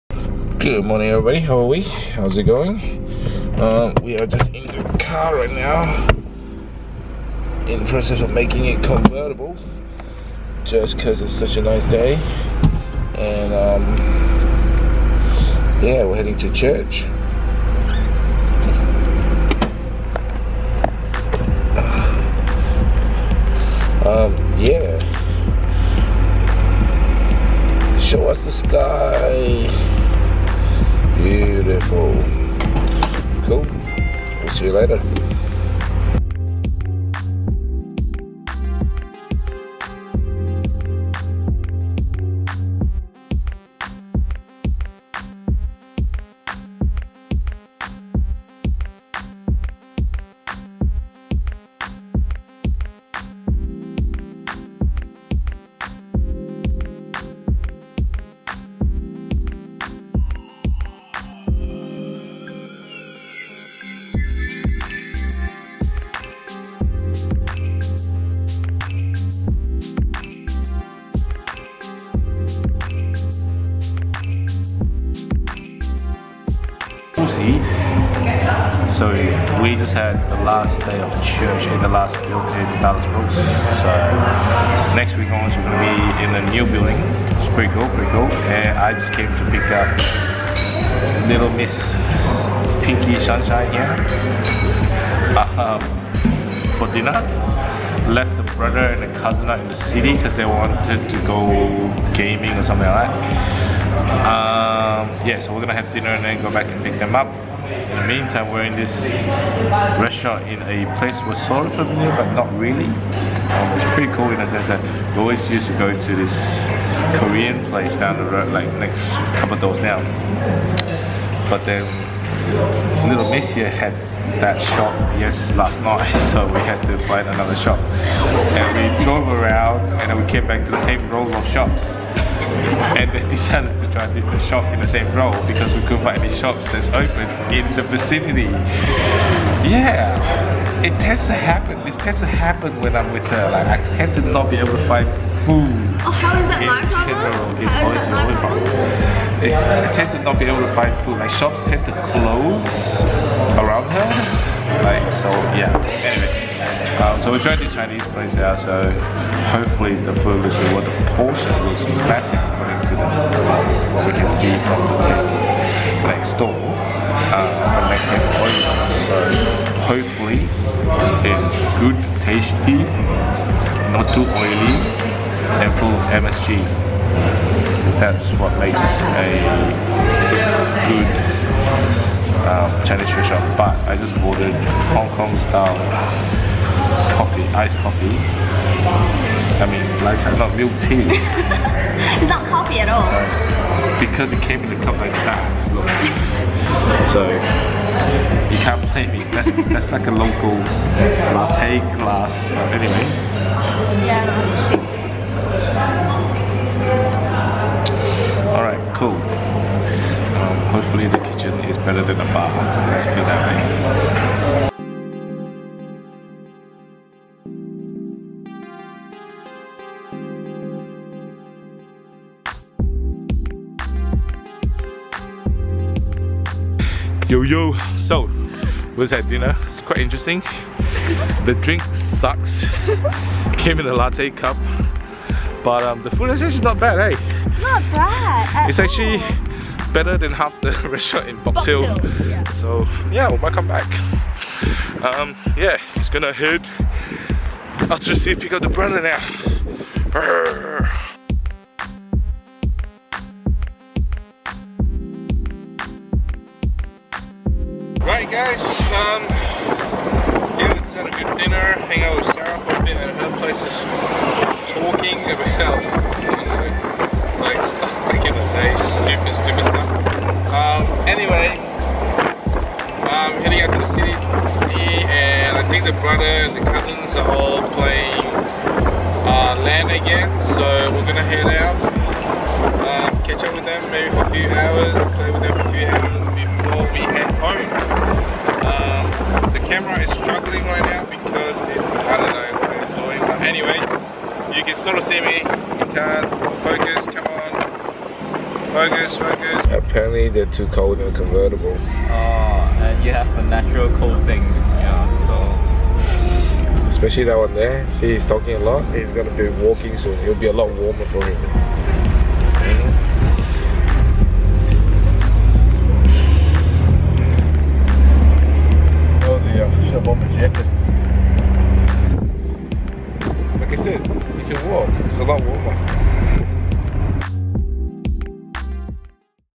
Church Party
VLog Episode 31 | Church Party

End of an era for Planetshakers having church weekly at Dallas Brooks Centre. We now have our own new building in Southbank, and today was a celebration for what we've done in this building for the pass 9 years and we Thank God for that. Can't wait to see the new church thou!!!

Or for what I do for a living;

Cheers!